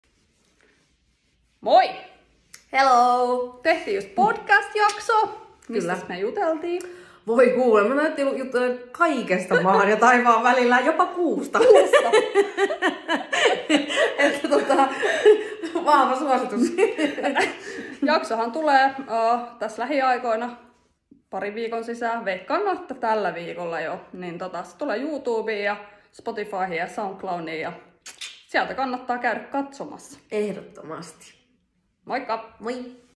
Sini Sabotage kävi vieraana SUPER J PODCASTISSA 🎙
Sini Sabotage kävi Super J Podcastissa vieraana 🎙

Jakso tulee unos lähiaikoina 🔥